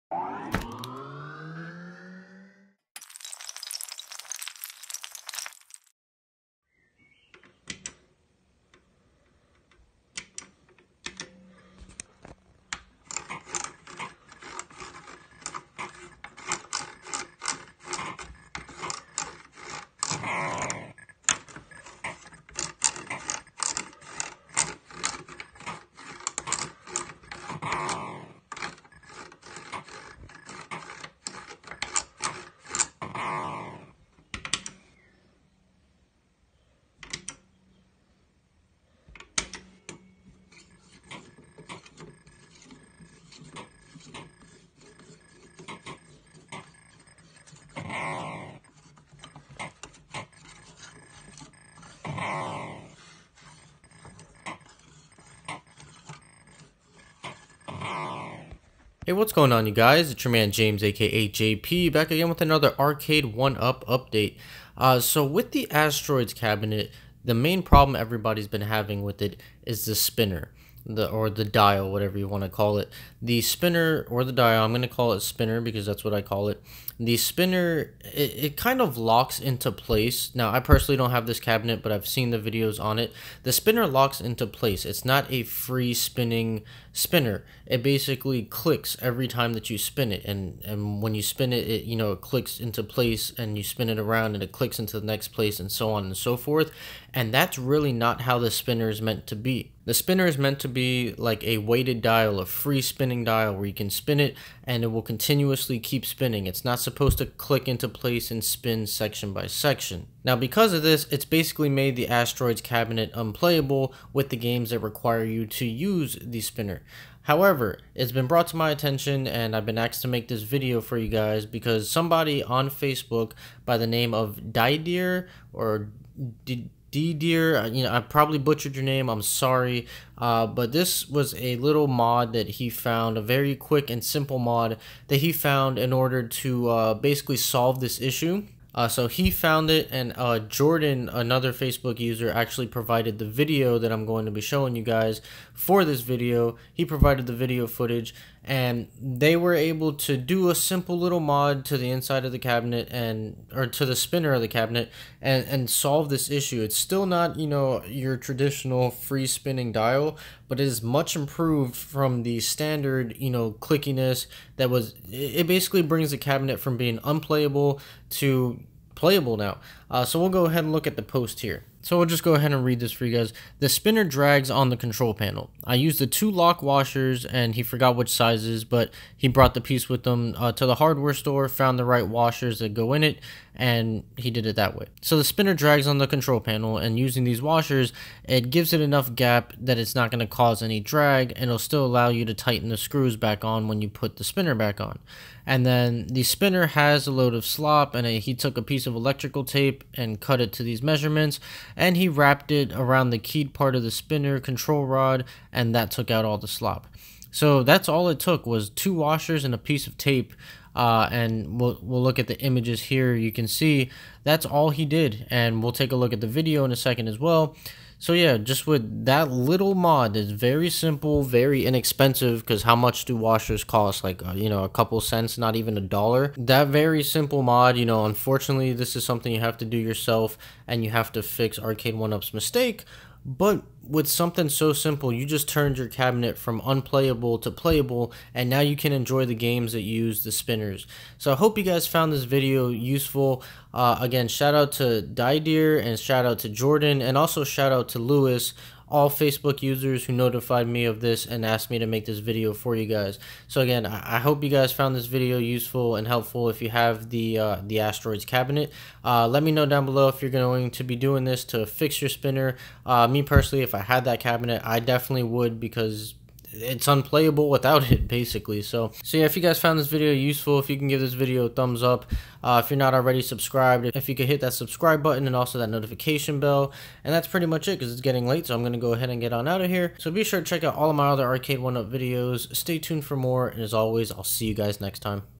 Arcade1Up Asteroids Spinner Issues FIXED (QUICK AND CHEAP MOD)
Hey guys! Today we're taking a look at how to fix the issues with the Arcade1Up Asteroids spinner dial! Hopefully this video helps you guys make your Asteroids cabinet playable now!